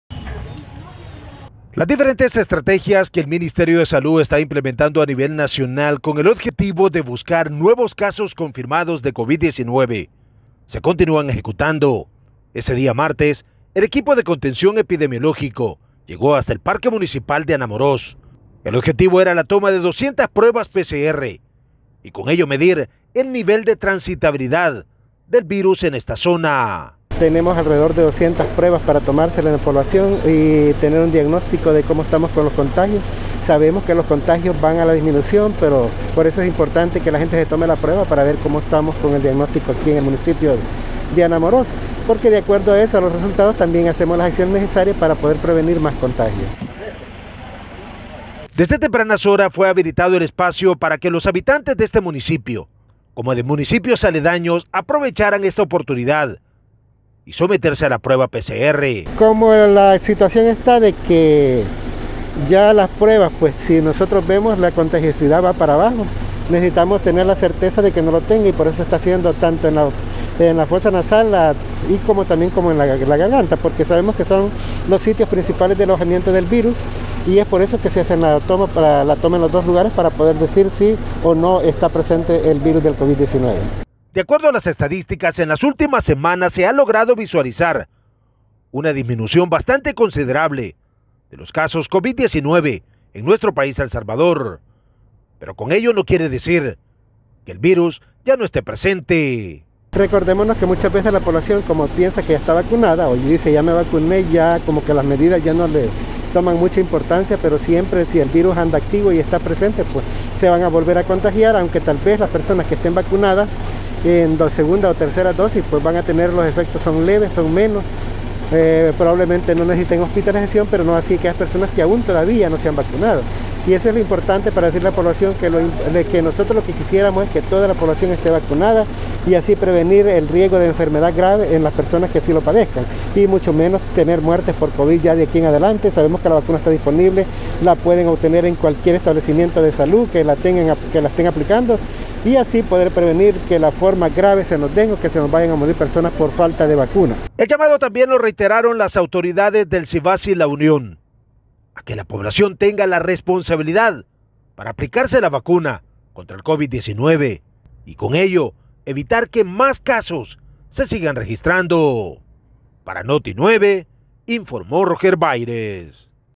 TAMIZAJE EN ANAMOROS
El Ministerio de Salud, en la lucha de combatir y detectar a tiempo el covid-19, sigue ejecutando a nivel nacional jornadas de tamizajes comunitarios, una de estas jornadas fue realizada en el municipio de Anamoròs.